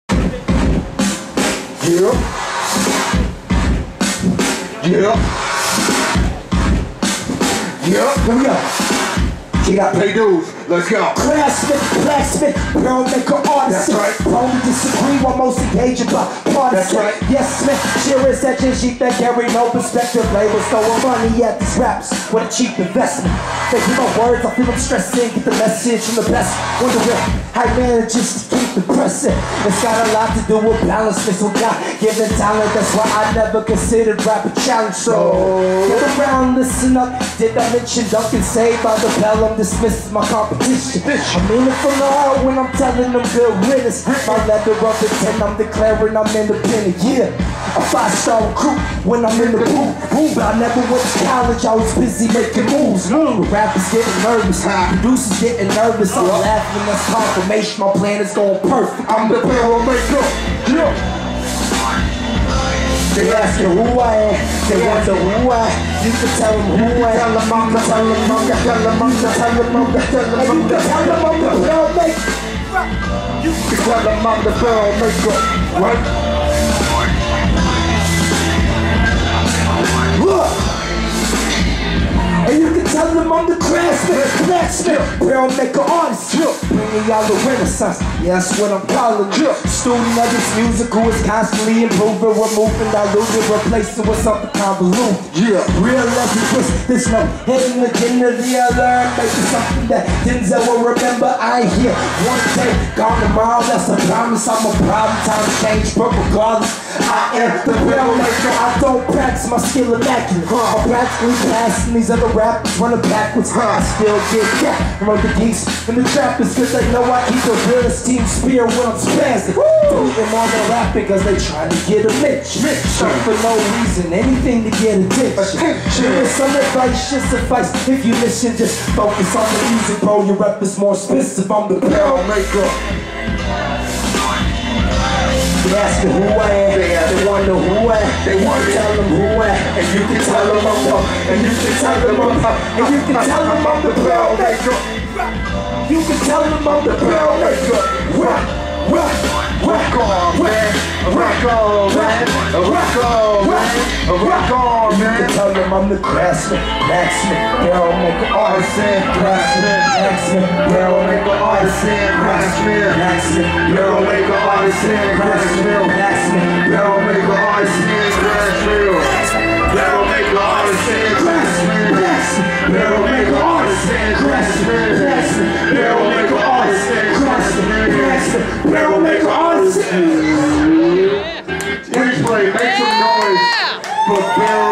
Barrel Maker at the Replay
Barrel Maker performs at the Replay in Lawrence, Kansas, on July 25, 2015.